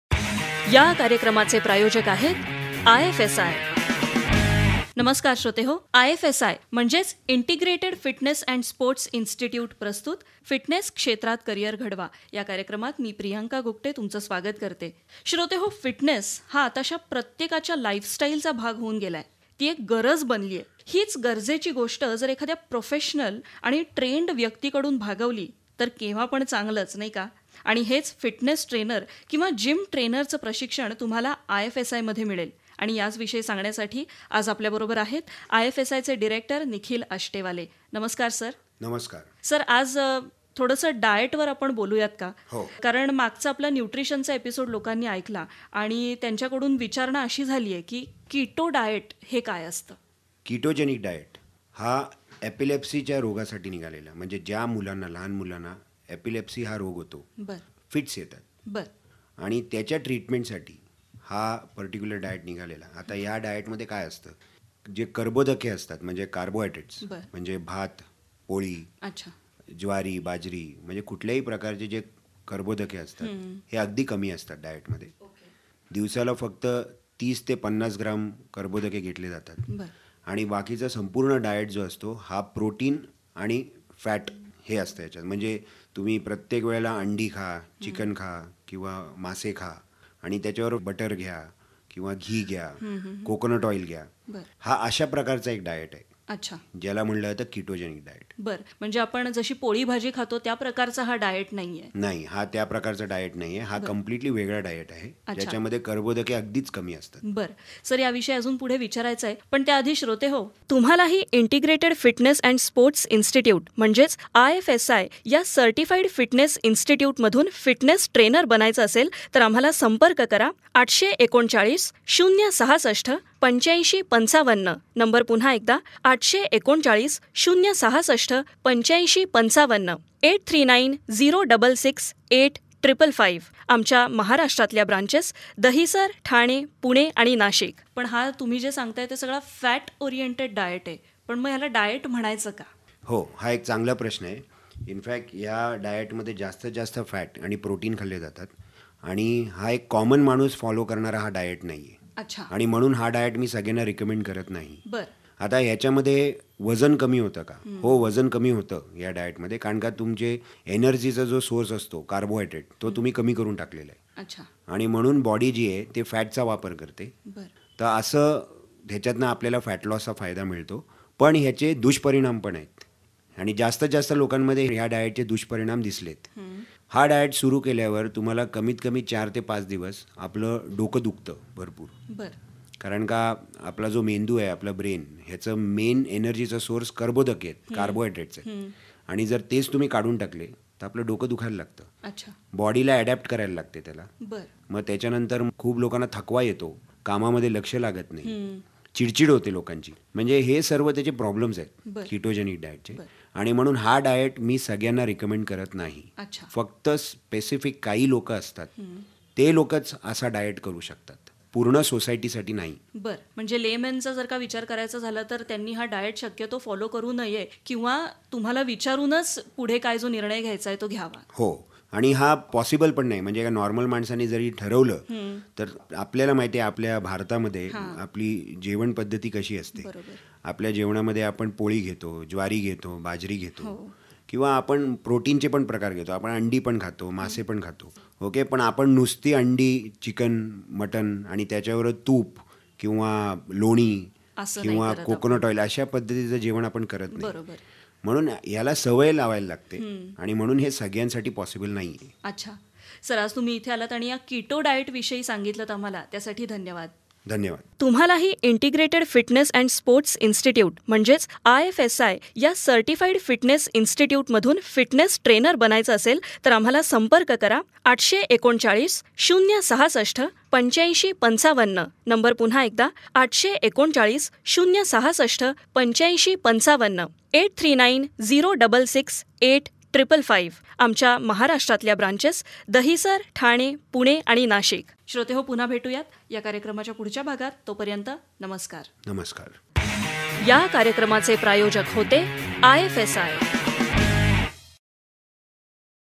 Radio Programme by IFSI - Episode 12
On building an outstanding career in fitness Industry. By Nikhil Ashtewale